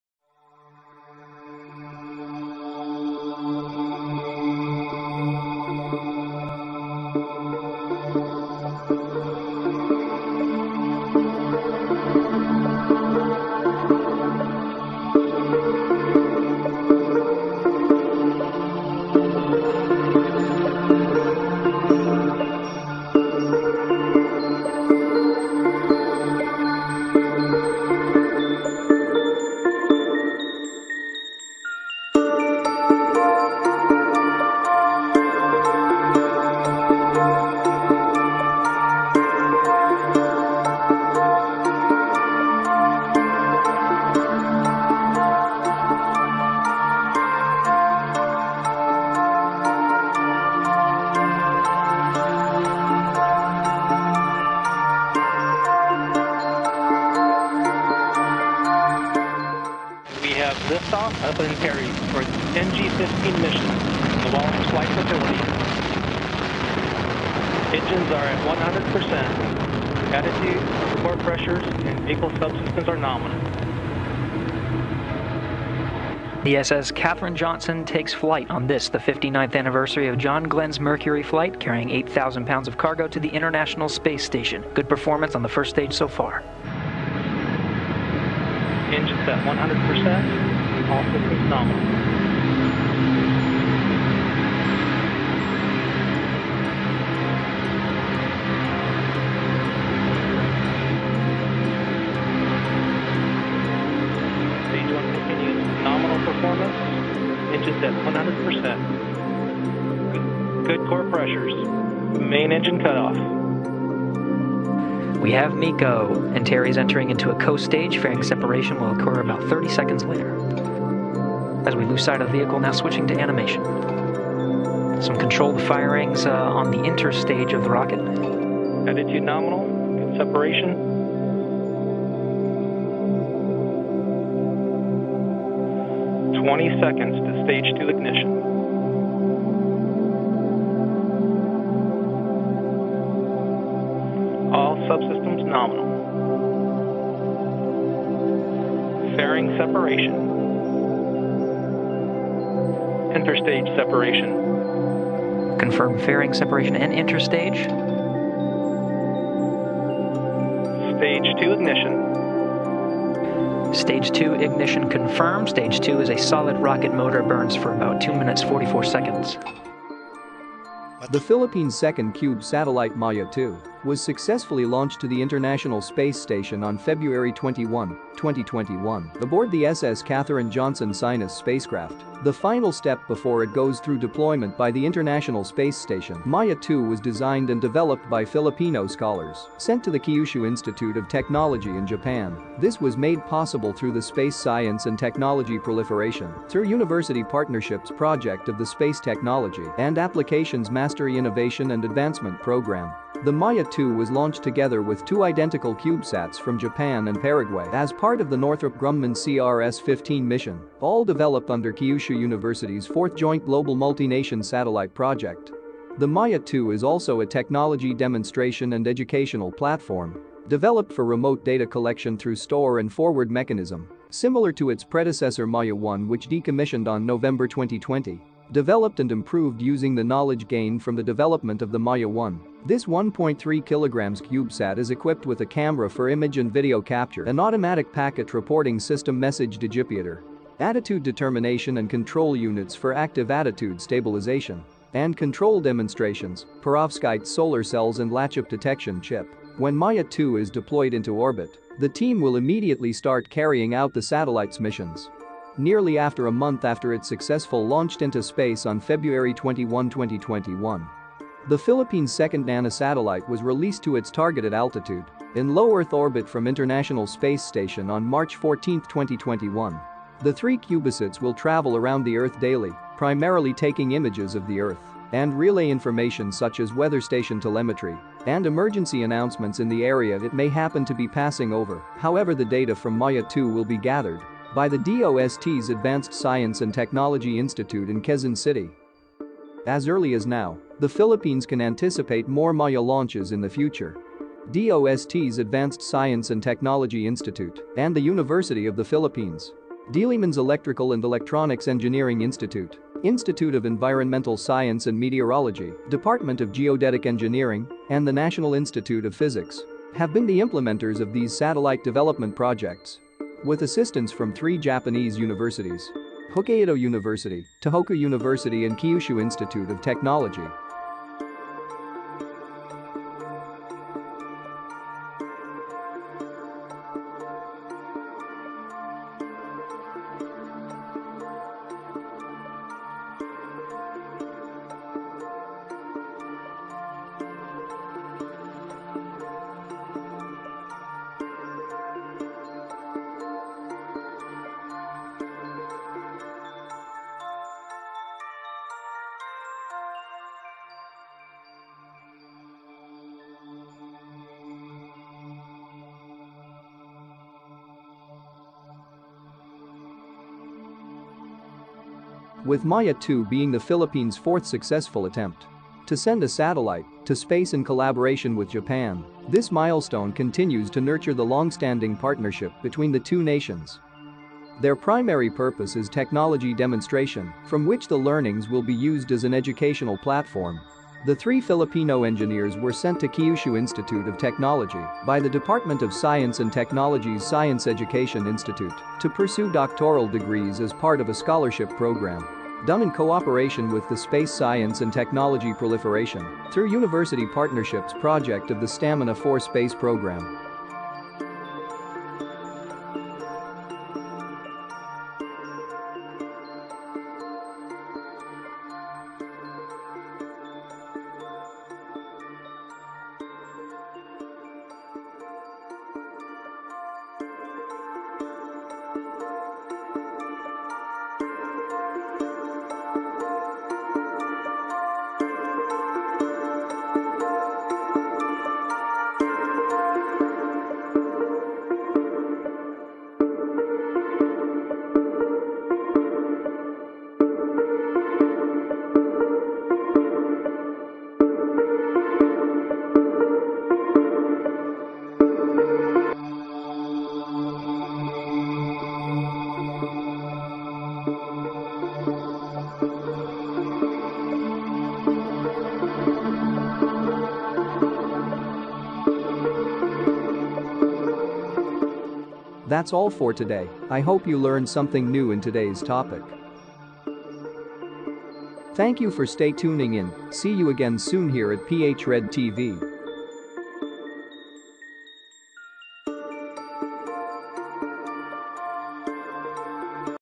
Philippines nanosattelite Maya 2 | launched in the International Space Station| DOST ASTI PH RED TV
PH nanosatellite Maya-2 now orbiting Earth after going to space last February 21, 2021

Nearly after a month after it successfully launched into space, the Philippines’ second nanosatellite is now orbiting the Earth.

Maya-2, which was designed by three Filipino engineers, was sent to the International Space Station (ISS) on Feb. 21

Maya-2 was deployed into space with two other cube satellites (cubesats) from Paraguay and Japan. All three are part of Kyushu Institute of Technology’s (Kyutech) 4th Joint Global Multi-Nation Birds Satellite

MAYA-2 Engineers:

Izrael IZNAR C Bautista - University of the Philippines
Marloun Sejera - Mapúa University
Mark angelo Purio - DE La Salle University / Adamson University

Other Satellites sent by THE Philippines to space are:
Diwata 1
Diwata 2
Maya 1
Maya 2

STAMINA4Space